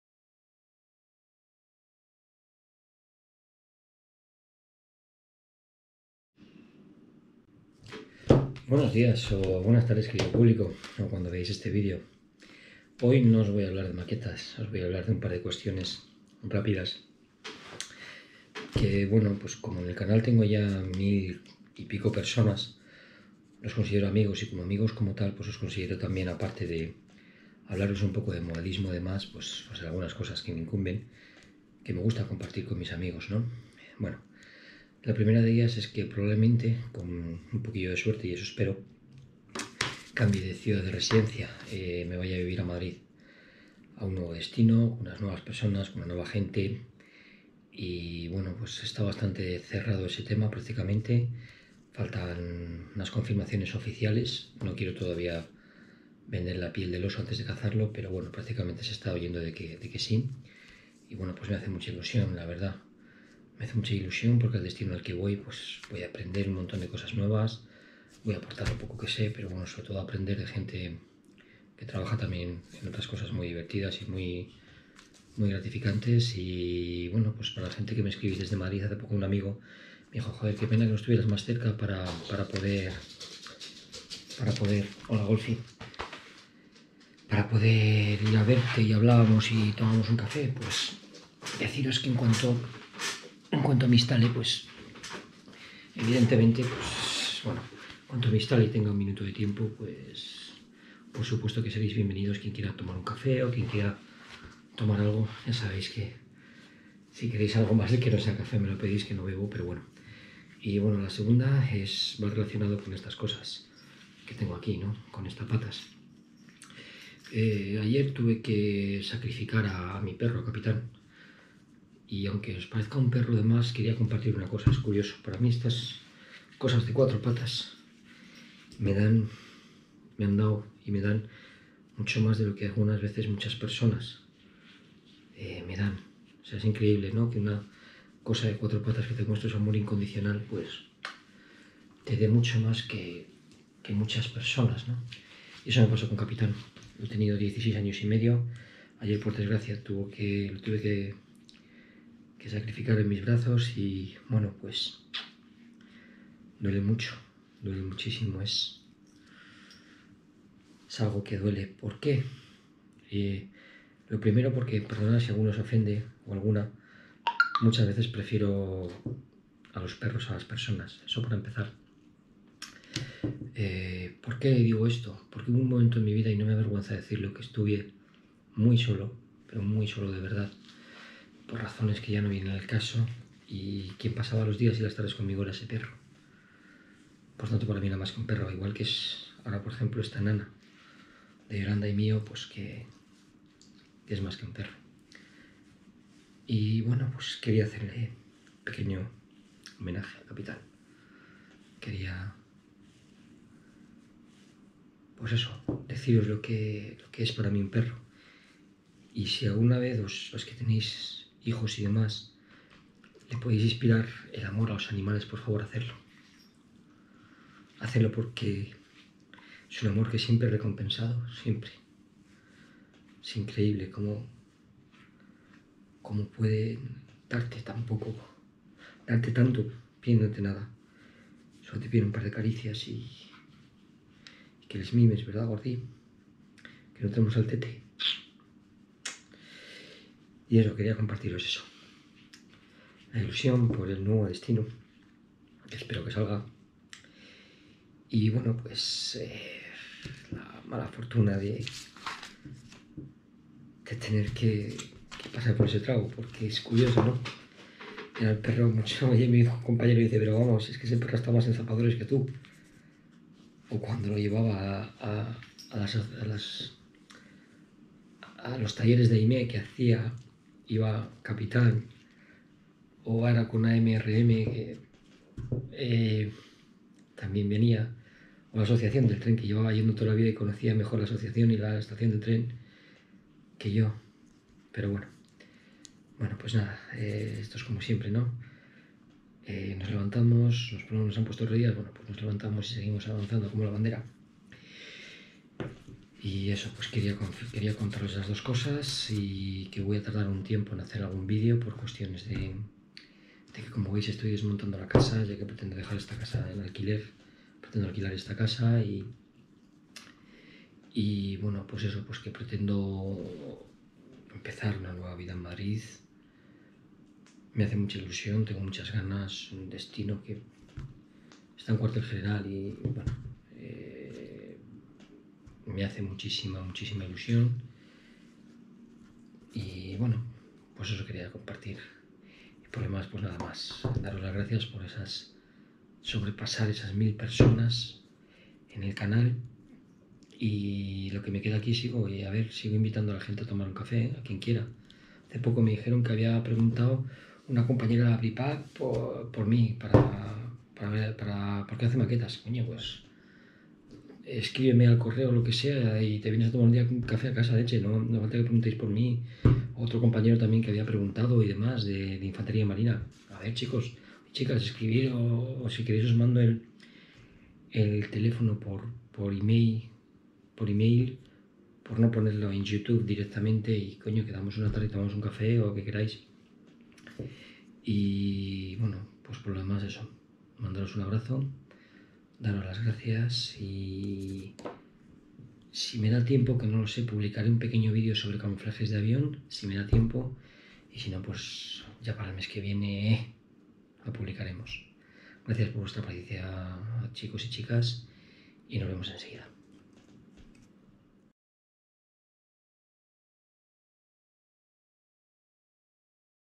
Agradecer los mas de 1000 suscriptores y un par de news
A todos vosotros y vosotras el agradecimiento por estar al otro lado y un par de noticias que quiero compartir